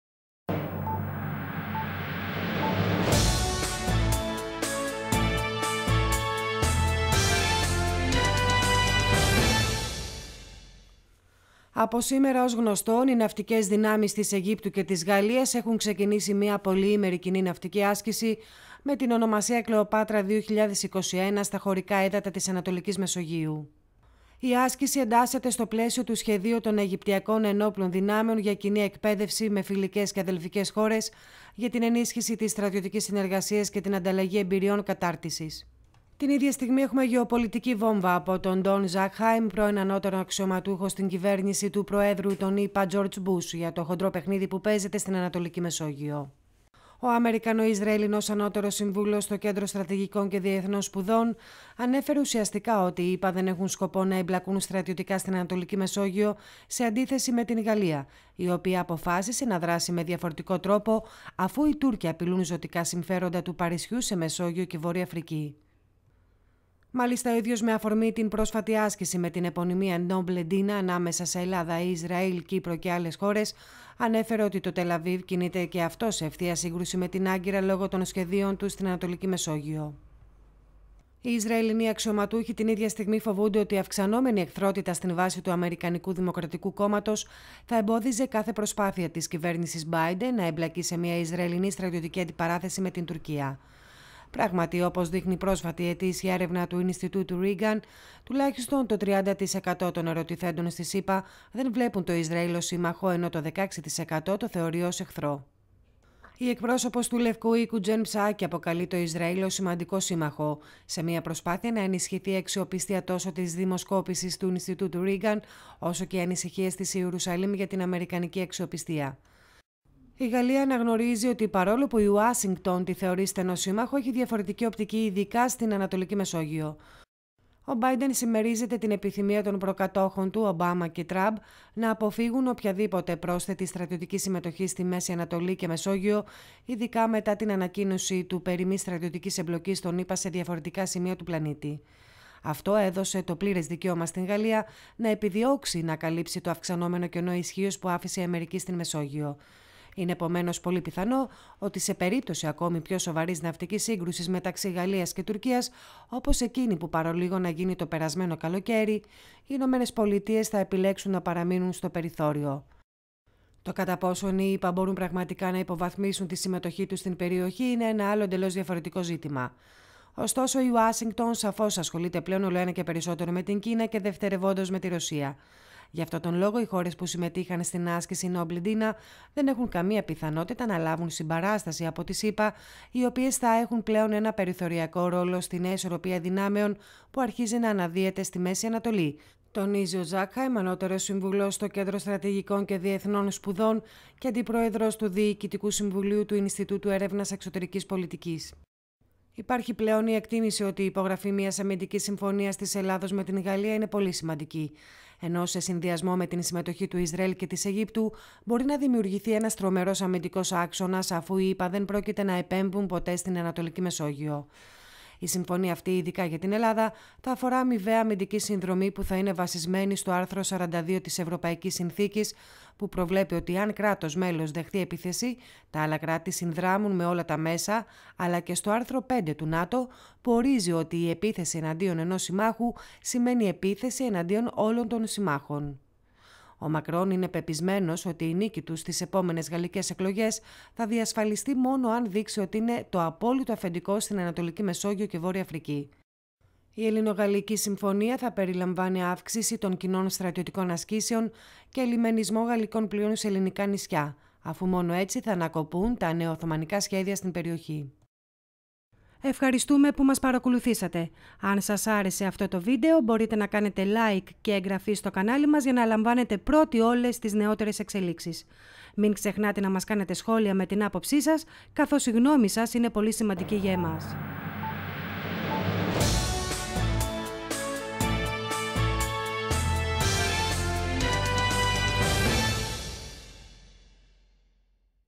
''Η Γαλλία θα εμπλακεί στρατιωτικά στην Α. Μεσόγειο''
Η Α.Μεσόγειος πεδίο σύγκρουσης Γάλλων-Ελλήνων-Αιγυπτίων-Ισραηλινών με τους Τούρκους
Από σήμερα ως γνωστόν, οι ναυτικές δυνάμεις της Αιγύπτου και της Γαλλίας έχουν ξεκινήσει μια πολυήμερη κοινή ναυτική άσκηση, με την ονομασία "Κλεοπάτρα 2021", στα χωρικά ύδατα της Α.Μεσογείου .

Η άσκηση εντάσσεται στο πλαίσιο του σχεδίου των Αιγυπτιακών Ένοπλων Δυνάμεων για κοινή εκπαίδευση με φιλικές και αδελφικές χώρες για την ενίσχυση της στρατιωτικής συνεργασίας και την ανταλλαγή εμπειριών κατάρτισης.

Την ίδια στιγμή έχουμε γεωπολιτική “βόμβα” από τον Dov Zakheim, πρώην ανώτερο αξιωματούχο στην κυβέρνηση του προέδρου των ΗΠΑ Τζορτζ Μπους, για το “χοντρό” παιχνίδι που παίζεται στην Α.Μεσόγειο.
Ο αμερικανο-Ισραηλινός ανώτερος σύμβουλος στο Κέντρο Στρατηγικών και Διεθνών Σπουδών, ανέφερε ουσιαστικά ότι οι ΗΠΑ δεν έχουν σκοπό να εμπλακούν στρατιωτικά στην Α.Μεσόγειο, σε αντίθεση με την Γαλλία, η οποία αποφάσισε να δράσει με διαφορετικό τρόπο αφού οι Τούρκοι απειλούν ζωτικά συμφέροντα του Παρισιού, σε Μεσόγειο και Β.Αφρική.
Μάλιστα ο ίδιος με αφορμή την πρόσφατη άσκηση με την επωνυμία Noble Dina ανάμεσα σε Ελλάδα, Ισραήλ, Κύπρο κά, ανέφερε ότι το Τελ Αβίβ κινείται και αυτό σε ευθεία σύγκρουση με την Άγκυρα λόγω των σχεδίων του στην Α.Μεσόγειο.
ΟΙ Ισραηλινοί αξιωματούχοι την ίδια στιγμή φοβούνται ότι η αυξανόμενη εχθρότητα στην βάση του αμερικανικού Δημοκρατικού Κόμματος θα εμπόδιζε κάθε προσπάθεια της κυβέρνησης Μπάιντεν να εμπλακεί σε μια ισραηλινή στρατιωτική αντιπαράθεση με την Τουρκία.
Πράγματι, όπως δείχνει η πρόσφατη ετήσια έρευνα του Ινστιτούτου Reagan, τουλάχιστον το 30 τοις εκατό των ερωτηθέντων στις ΗΠΑ, δεν βλέπουν το Ισραήλ ως σύμμαχο ενώ το 16 τοις εκατό το θεωρεί ως εχθρό.
Η εκπρόσωπος του Λευκού Οίκου Τζεν Ψάκη αποκαλεί το Ισραήλ ως σημαντικό σύμμαχο, σε μια προσπάθεια να ενισχυθεί η αξιοπιστία τόσο της δημοσκόπησης του Ινστιτούτου Reagan. όσο και οι ανησυχίες της Ιερουσαλήμ για την αμερικανική αξιοπιστία.

“Η Γαλλία αναγνωρίζει ότι παρόλο που η Ουάσινγκτον τη θεωρεί στενό σύμμαχο έχει διαφορετική οπτική ειδικά στην Α.Μεσόγειο.

Ο Μπάιντεν συμμερίζεται την επιθυμία των προκατόχων του (Ομπάμα και Τραμπ) να αποφύγουν οποιαδήποτε πρόσθετη στρατιωτική συμμετοχή στη Μέση Ανατολή και Μεσόγειο, ειδικά  μετά την ανακοίνωση του περί μη στρατιωτικής εμπλοκής των ΗΠΑ σε διάφορα σημεία του πλανήτη.
Αυτό έδωσε το πλήρες δικαίωμα στην Γαλλία να επιδιώξει να καλύψει το αυξανόμενο κενό ισχύος που άφησε η Αμερική στην Μεσόγειο.
Είναι, επομένως, πολύ πιθανό ότι σε περίπτωση ακόμη πιο σοβαρής ναυτικής σύγκρουσης μεταξύ Γαλλίας και Τουρκίας, όπως εκείνη που παρολίγον να γίνει το περασμένο καλοκαίρι, οι Ηνωμένες Πολιτείες θα επιλέξουν να παραμείνουν στο περιθώριο.
Το κατά πόσον οι ΗΠΑ μπορούν πραγματικά να υποβαθμίσουν τη συμμετοχή τους στην περιοχή, είναι ένα εντελώς διαφορετικό ζήτημα.
Ωστόσο, η Ουάσιγκτον σαφώς ασχολείται πλέον ολοένα και περισσότερο με την Κίνα και δευτερευόντως με τη Ρωσία.
Για αυτόν τον λόγο οι χώρες που συμμετείχαν στην άσκηση  Noble Dina δεν έχουν καμία πιθανότητα να λάβουν συμπαράσταση από τις Ηνωμένες Πολιτείες, οι οποίες θα έχουν πλέον έναν περιθωριακό ρόλο στη νέα ισορροπία δυνάμεων που αρχίζει να αναδύεται στη Μέση Ανατολή”, τονίζει ο Dov S. Zakheim ανώτερος σύμβουλος στο Κέντρο Στρατηγικών και Διεθνών Σπουδών και αντιπρόεδρος του διοικητικού συμβουλίου του Ινστιτούτου Έρευνας Εξωτερικής Πολιτικής.
Υπήρξε υφυπουργός άμυνας (ελεγκτής) και διευθύνων σύμβουλος του υπουργείου άμυνας των ΗΠΑ από το 2001 έως το 2004 και αναπληρωτής υφυπουργός άμυνας από το 1985 έως το 1987.
Υπάρχει πλέον η εκτίμηση ότι η υπογραφή μιας αμυντικής συμφωνίας της Ελλάδος με την Γαλλία είναι πολύ σημαντική , ενώ σε συνδυασμό με την συμμετοχή του Ισραήλ και της Αιγύπτου, μπορεί να δημιουργηθεί ένας τρομερός αμυντικός άξονας, αφού οι ΗΠΑ δεν πρόκειται να επέμβουν ποτέ στην Α.Μεσόγειο.
Η συμφωνία αυτή ειδικά για την Ελλάδα, θα αφορά αμοιβαία αμυντική συνδρομή που θα είναι βασισμένη στο άρθρο 42 της ευρωπαϊκής συνθήκης που προβλέπει ότι αν κράτος μέλος δεχθεί επίθεση τα άλλα κράτη συνδράμουν με όλα τα μέσα αλλά και στο άρθρο 5 του ΝΑΤΟ  που ορίζει ότι η επίθεση εναντίον ενός συμμάχου σημαίνει επίθεση εναντίον όλων των συμμάχων.
Ο Μακρόν είναι πεπεισμένος ότι η νίκη του στις επόμενες γαλλικές εκλογές θα διασφαλιστεί μόνο αν δείξει ότι  είναι το απόλυτο αφεντικό  στην Ανατολική Μεσόγειο και Βόρεια Αφρική. Η ελληνογαλλική συμφωνία θα περιλαμβάνει αύξηση των κοινών στρατιωτικών ασκήσεων και ελλιμενισμό γαλλικών πλοίων σε ελληνικά λιμάνια, αφού μόνο έτσι θα ανακοπούν τα νεο-οθωμανικά σχέδια στην περιοχή.